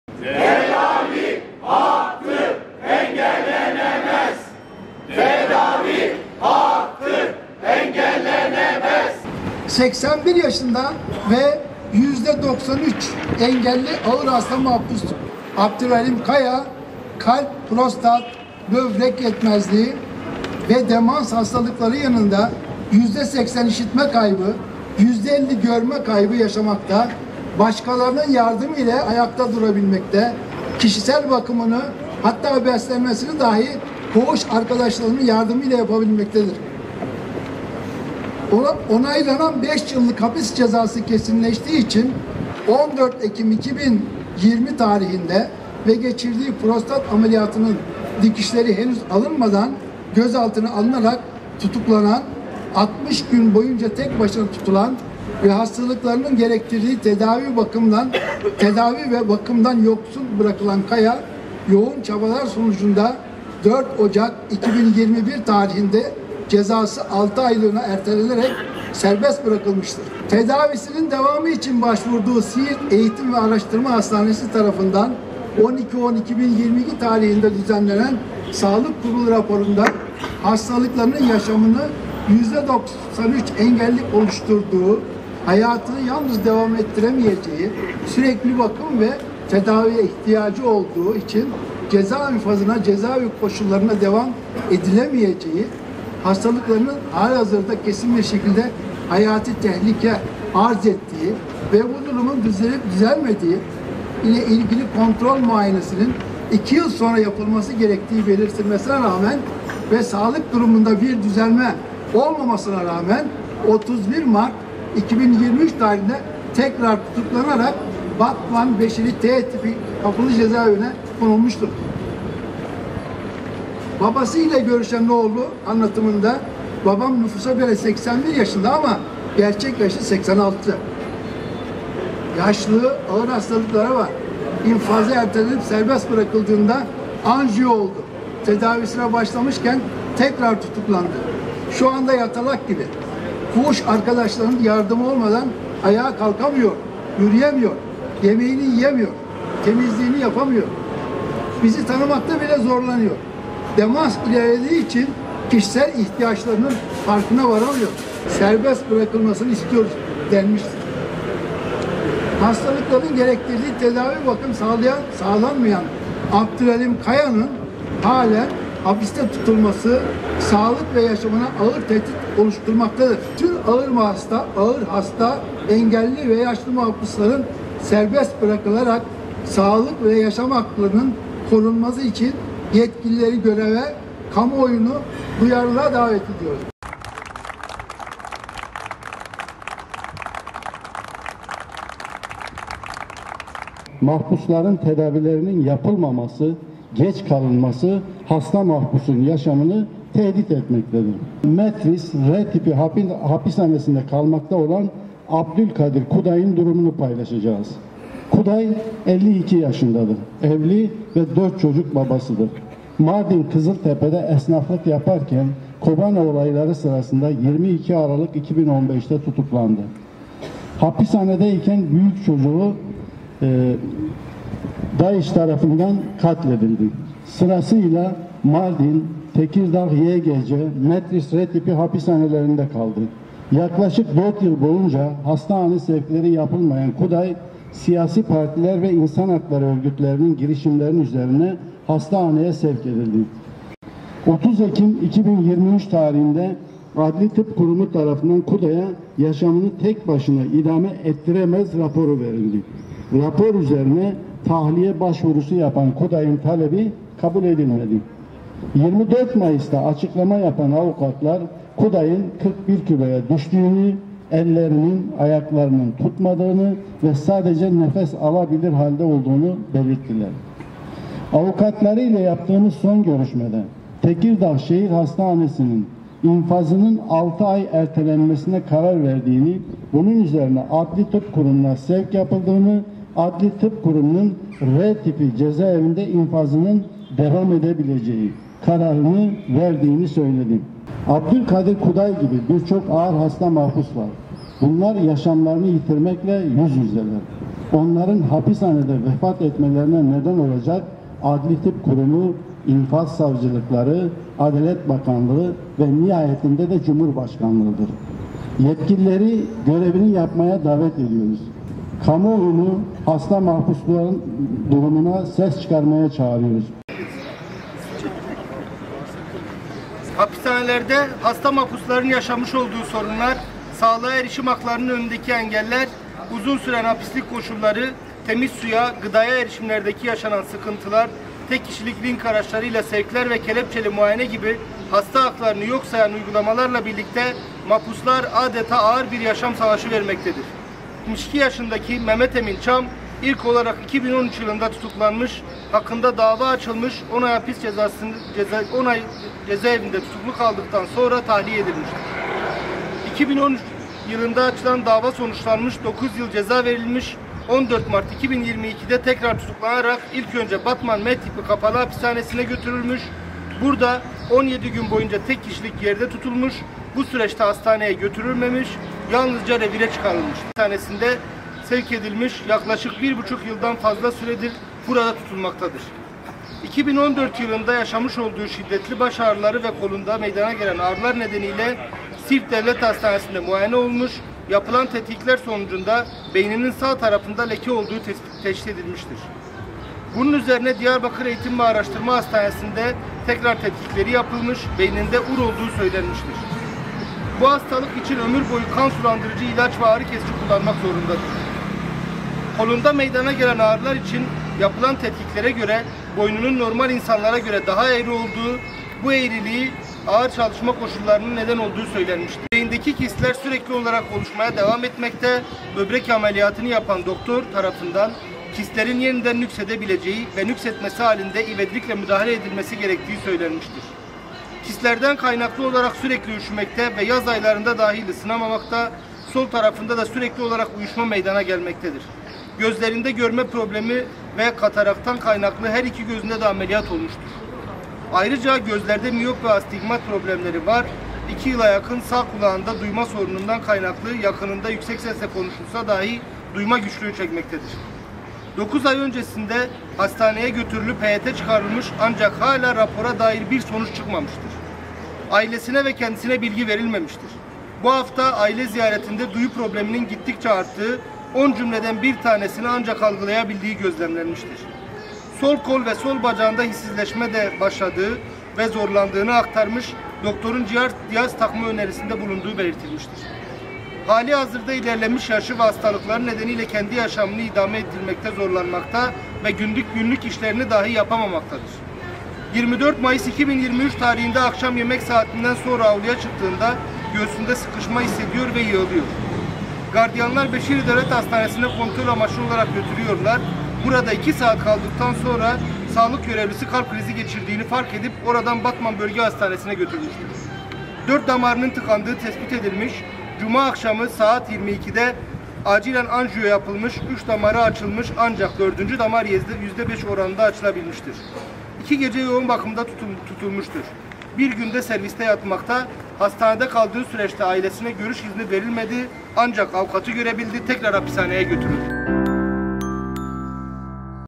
HASTA TUTSAKLAR KAYA, ÇAM VE KUDAY’IN TAHLİYESİ İSTENDİ
İstanbul, Ankara ve İzmir’de hasta tutsaklar için yapılan eylemlerde Abdulalim Kaya, Mehmet Emin Çam ve Abdülkadir Kuday’ın tahliyesi istendi.